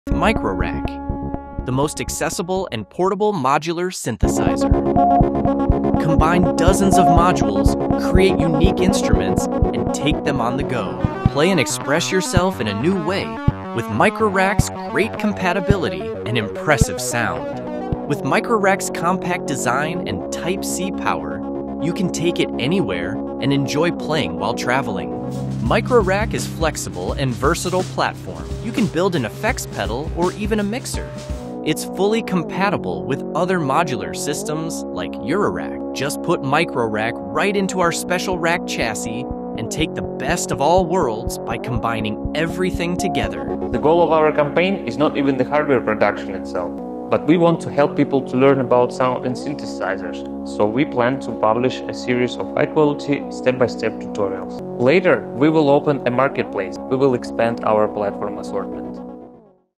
A Kickstarter Project We Love: Microrack - The Most Accessible Modular Synthesizer
MICRORACK: The Most Accessible Modular Synthesizer
MICRORACK is a small modular synthesizer right on the breadboard. It's great for playing, learning and has impressive sound variability